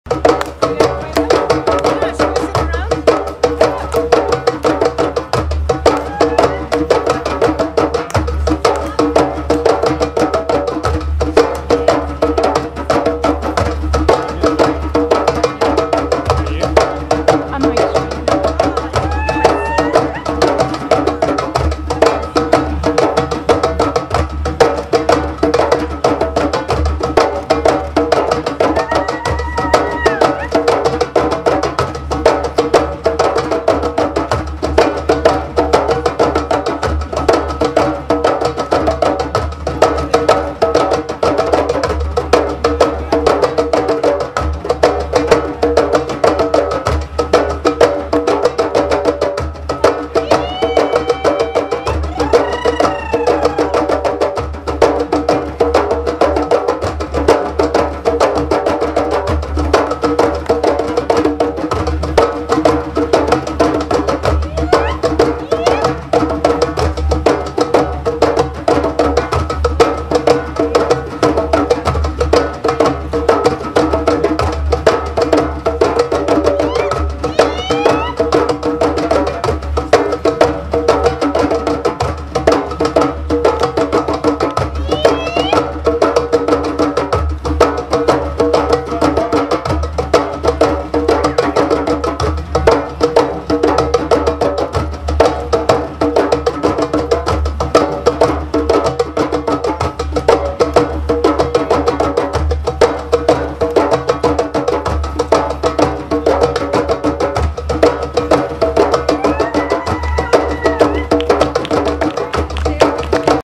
Maria's Basket Balance Dance
Performing with the amazing Firedance Drummers
at Treasureventure Fair 2012

June 22-24 at the Rockton Fairgrounds, Rockton Ontario Treasureventure 2012